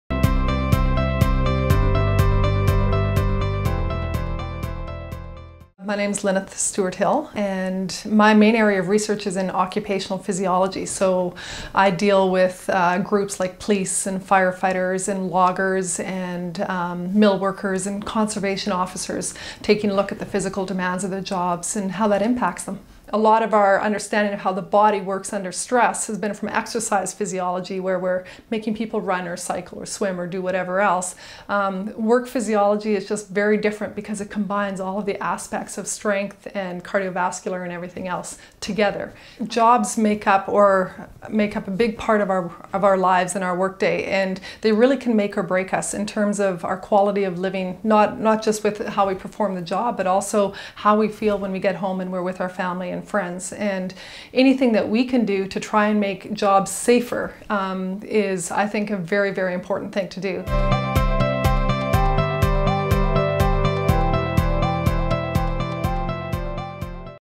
Faces of UVic Research: Lynneth Stuart Hill
Dr. Lynneth Stuart-Hill is an Assistant Professor with the School of Exercise Science, Physical and Health Education in the Faculty of Education. In this video, Lynneth talks about her research in occupational physiology, where she looks at the physical demands of jobs and how they affect the employee.